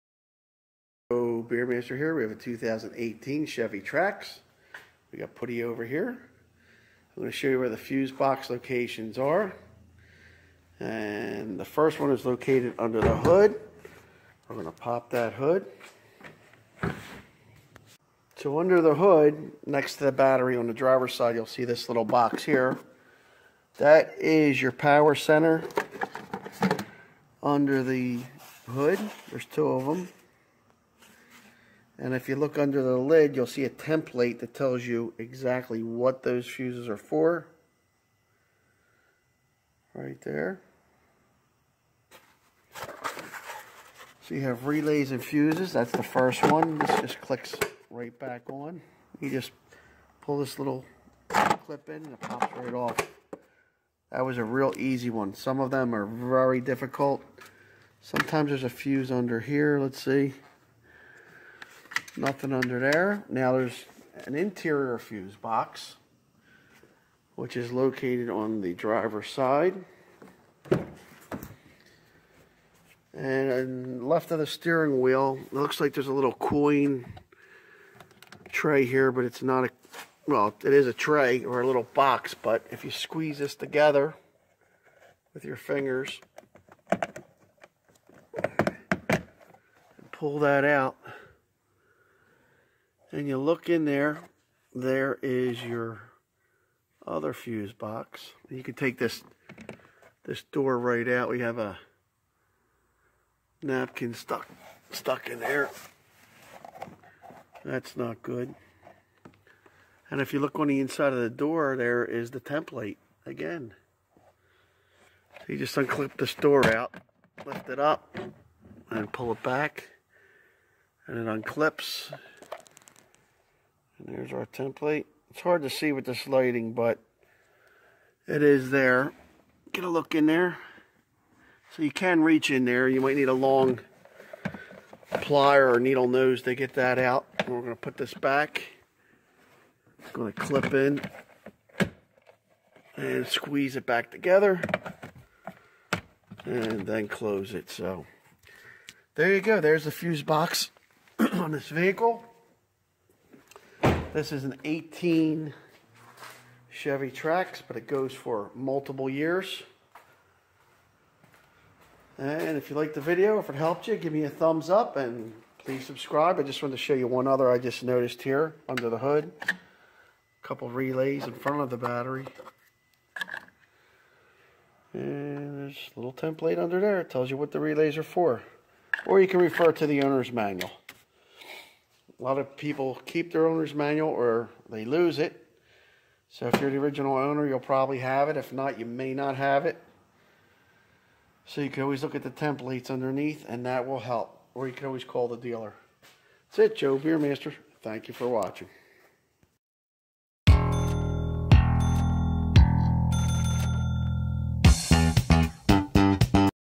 Chevy Trax & Buock Encore Fuse Box Locations 2012 to Present
Location of Chevy Trax & Buick Encore  fuse box locations. Note there is an additional hidden fuse box in left quarter panel behind the trim.  Must unscrew panels off to access.